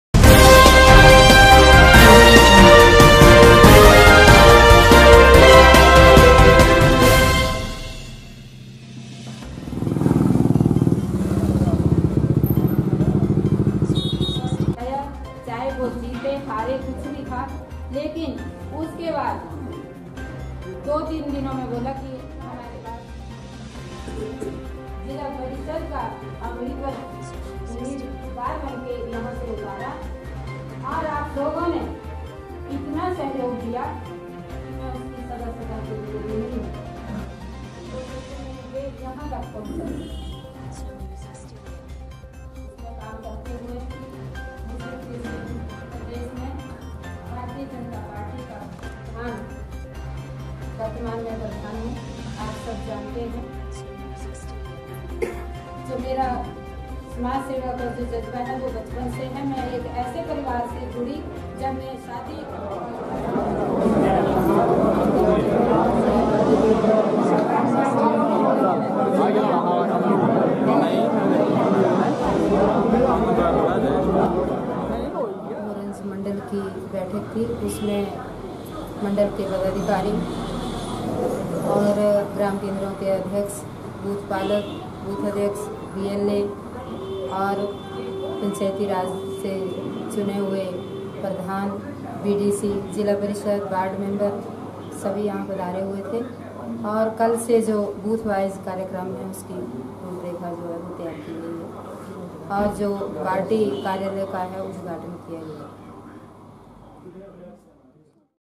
आज कमलेश कुमारी ने भोरंज के तर्कवाड़ी में पार्टी कार्यलय का किया उद्धघाटन।
Sm news